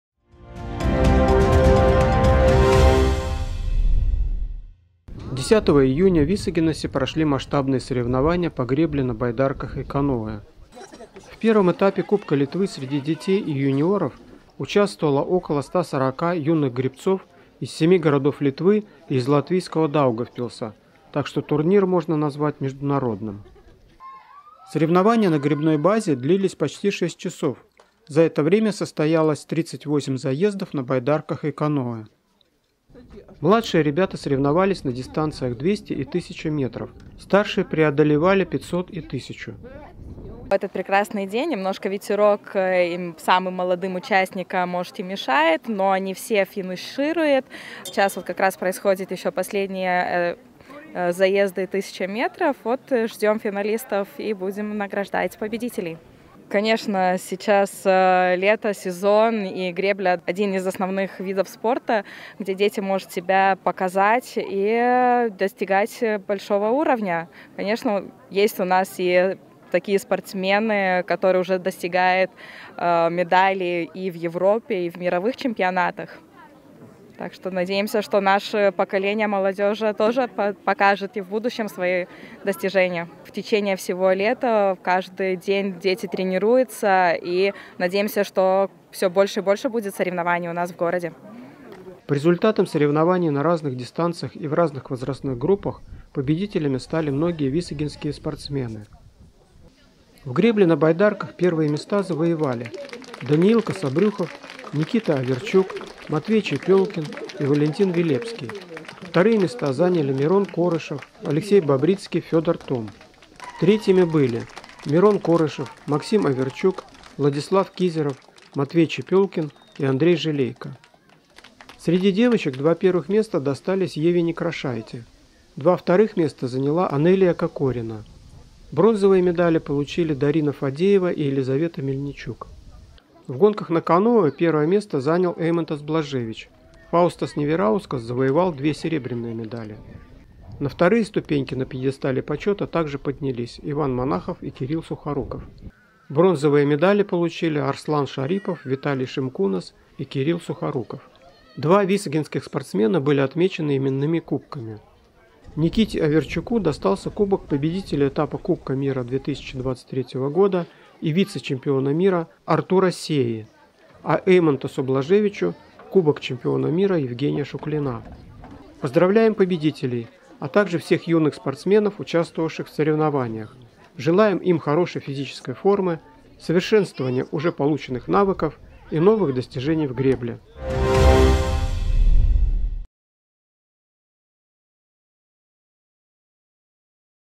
На висагинском озере кипели спортивные страсти
В первом этапе кубка Литвы среди детей и юниоров участвовало около 140 юных гребцов из семи городов Литвы и из латвийского Даугавпилса, так что турнир можно назвать международным.

Соревнования на гребной базе длились почти шесть часов, за это время состоялось 38 заездов на байдарках и каноэ. Младшие ребята соревновались на дистанциях 200 и 1000 метров, старшие преодолевали 500 и 1000 метров.

По результатам соревнований на разных дистанциях и в разных возрастных группах победителями стали многие висагинские спортсмены.

В гребле на байдарках первые места завоевали Даниил Кособрюхов, Никита Оверчук, Матвей Чепёлкин и Валентин Велебский. Вторые места заняли Мирон Корышев, Алексей Бобрицкий, Федор Томм. Третьими были: Мирон Корышев, Максим Оверчук, Владислав Кизеров, Матвей Чепёлкин и Андрей Жалейко.

Среди девочек два первых места досталось Еве Некрошайте. Два вторых места заняла Анелия Кокорина. Бронзовые медали получили Дарина Фадеева и Елизавета Мельничук.

В гонках на каноэ первое место занял Эймантас Блажевич. Фаустас Невераускас завоевал две серебряные медали. На вторые ступеньки на пьедестале почёта также поднялись Иван Монахов и Кирилл Сухоруков. Бронзовые медали получили Арслан Шарипов, Виталий Шимкунас и Кирилл Сухоруков.

Два висагинских спортсмена были отмечены именными кубками. Никите Оверчуку достался кубок победителя этапа Кубка мира 2023 г. и вице-чемпиона мира Артура Сеи, а Эймантасу Блажевичу – кубок чемпиона мира Евгения Шуклина.